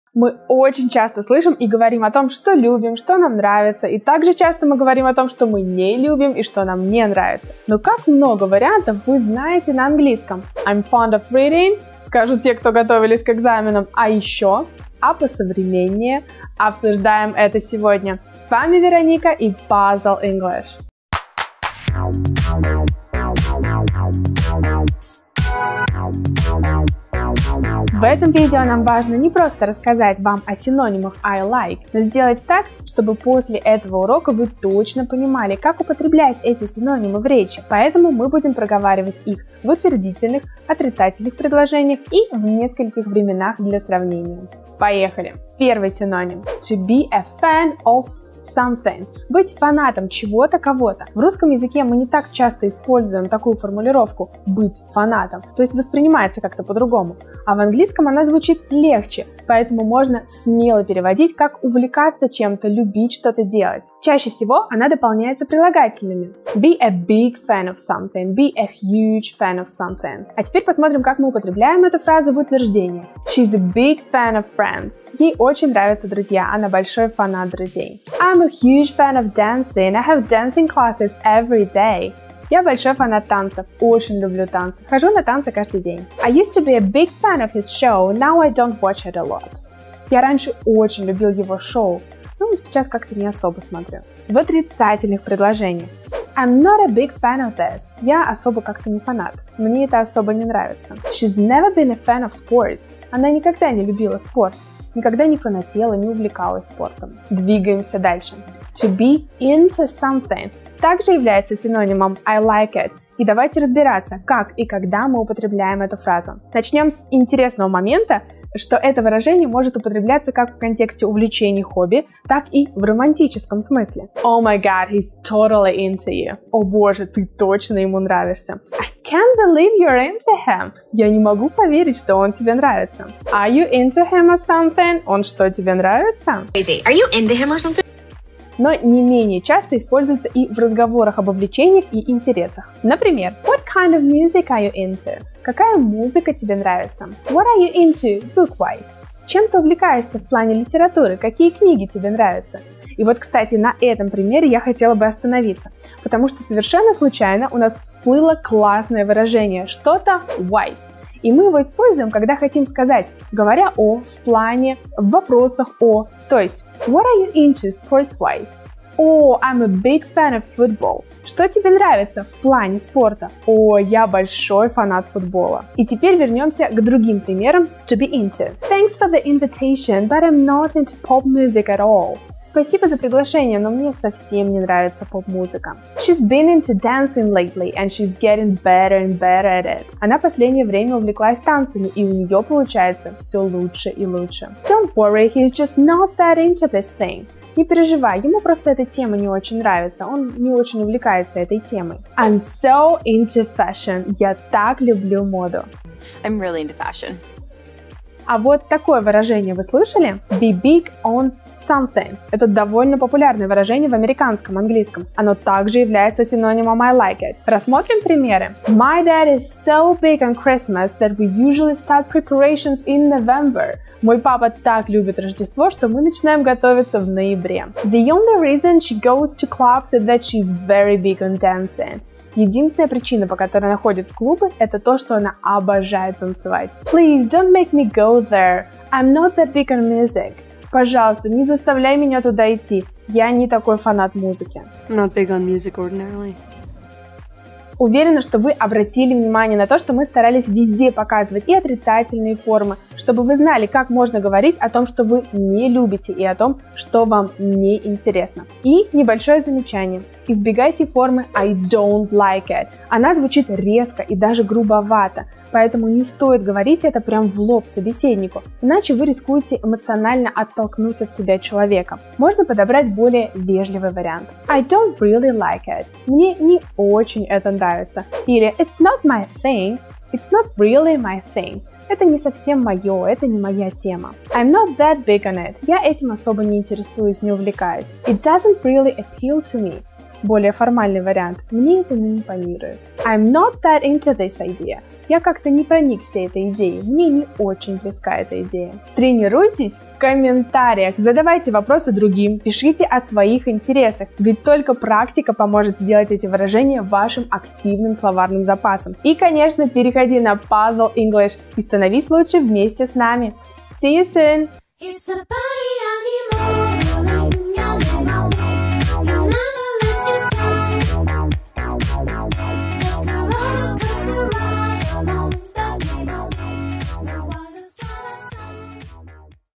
Рассказываем о том, что нам нравится или не нравится || Puzzle English
Мы очень часто слышим и говорим о том, что любим, что нам нравится и также часто о том, что мы не любим, что нам не нравится. Но как много вариантов вы знаете на английском? I’m fond of reading? Скажут те, кто готовились к экзаменам.
А ещё?
А посовременнее?
Обсуждаем сегодня. С вами Вероника и Puzzle English.
Поехали!

Учите английский по самой выгодной цене!

Занимайтесь с ментором и тренируйте речь. Запишитесь на пробный урок